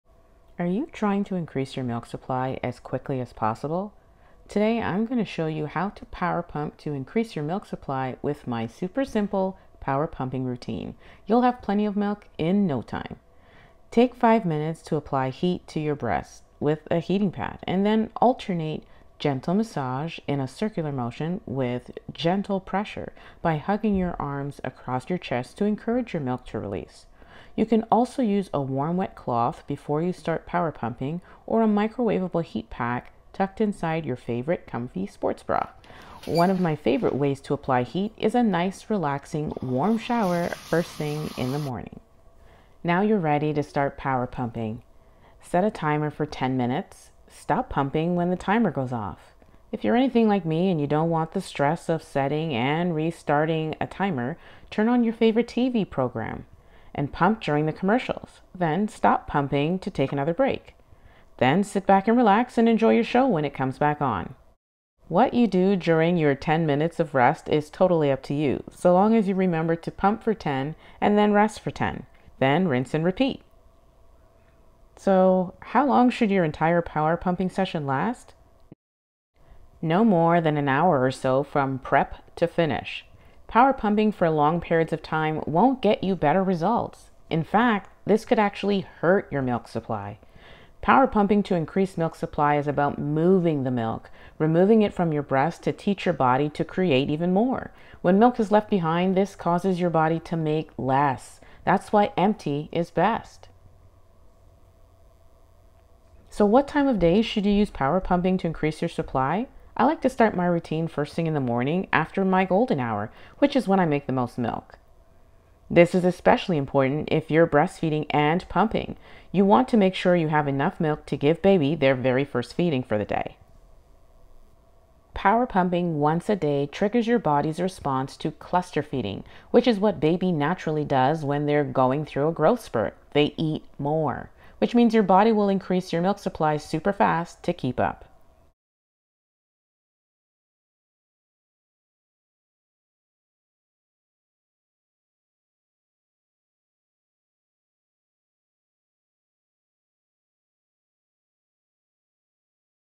Power Pumping Triples Supply...Let me explain.
Trying to increase your milk supply quickly?  Power pumping is one of the most powerful tools you can use to get your milk supply up fast.  Here are all my power pumping tips and a detailed power pumping schedule you can use to increase your milk supply now.

HOW TO INCREASE BREAST MILK SUPPLY QUICKLY | POWER PUMPING TIPS | POWER PUMPING SCHEDULE

Feeding Tube (NG)
Breast Pump
Bottles and Bags